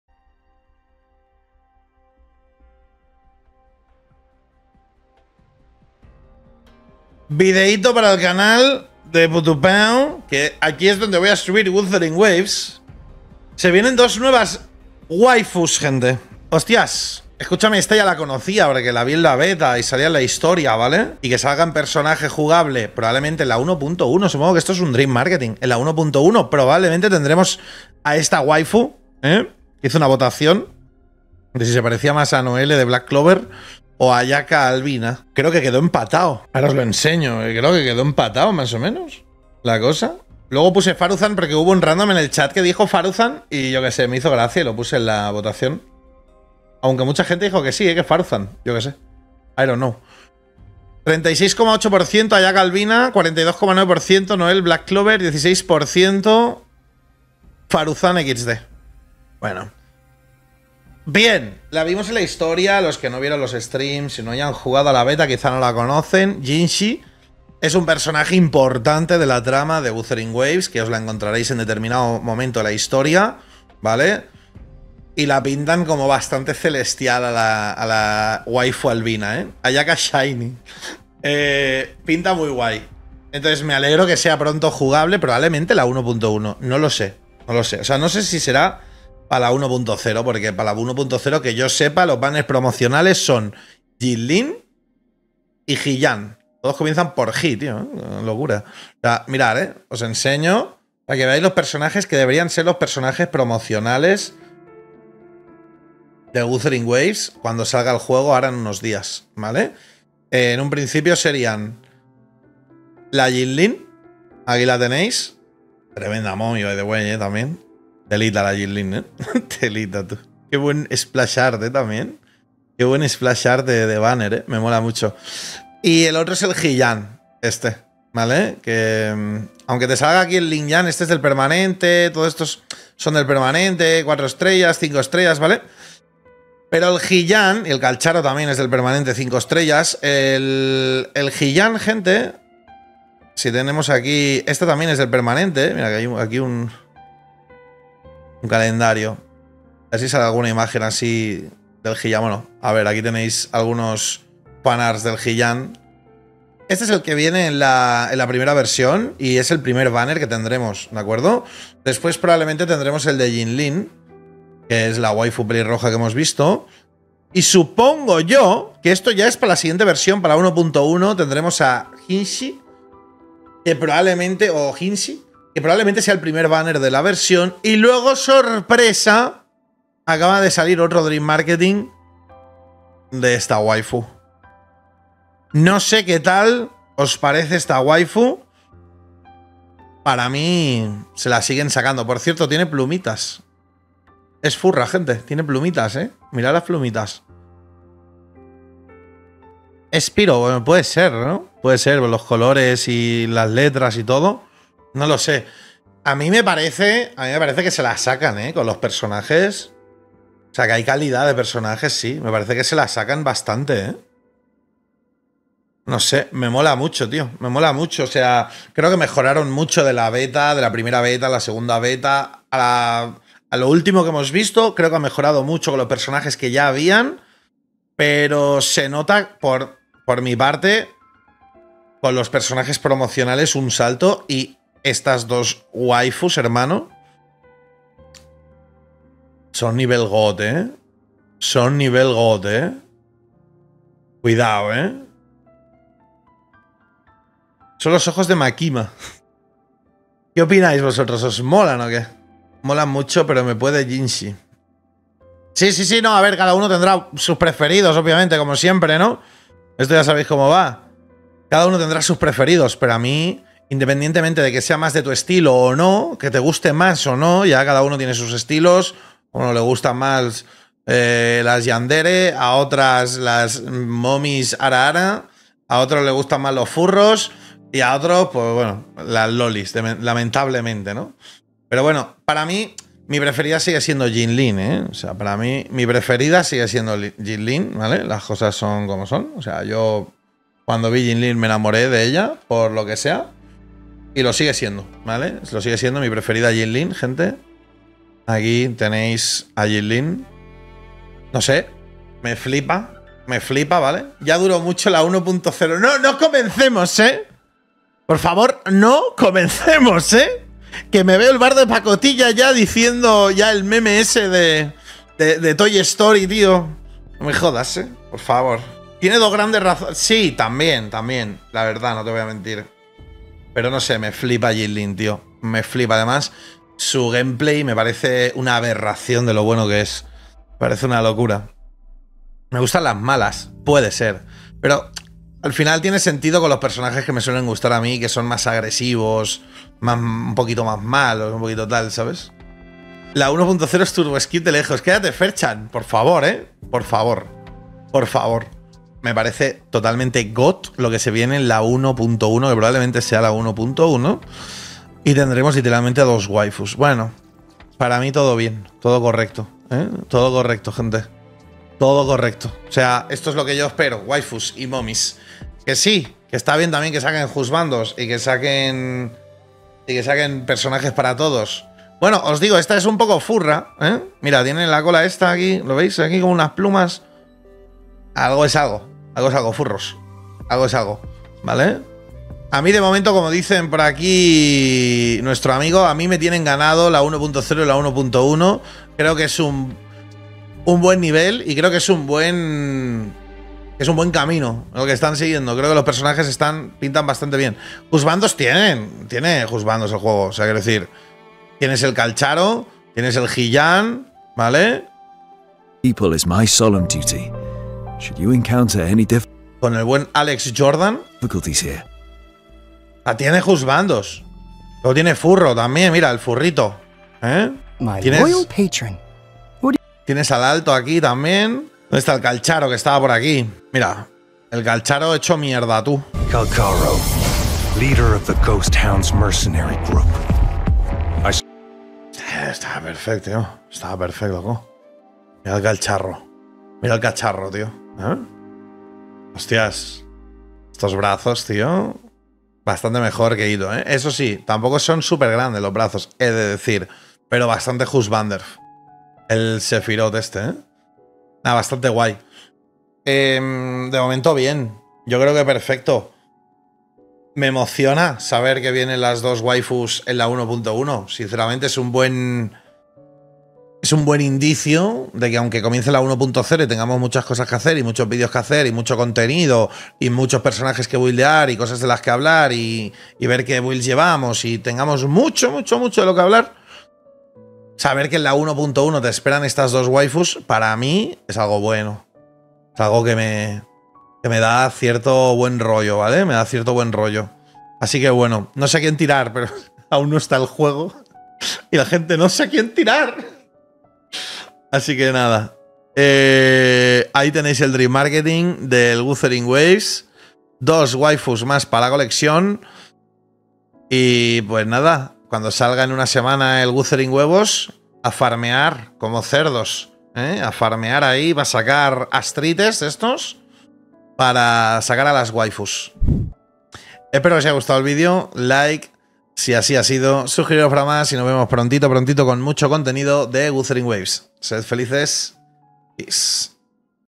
⚠️WUTHERING WAVES CADA VEZ MAS CERCA⭐¡JINHSI y CHANGLI NUEVOS PERSONAJES DE LA 1.1!
Familia, KUROGAMES acaba de mostrarnos por su Twitter oficial los dos nuevos personajes que se vendrán en la versión 1.1 que vendrá los primeros personajes en Wuthering Waves ¡Jinhsi y Changli!
Pre registrate para llevarte las recompensas de inicio del juego!!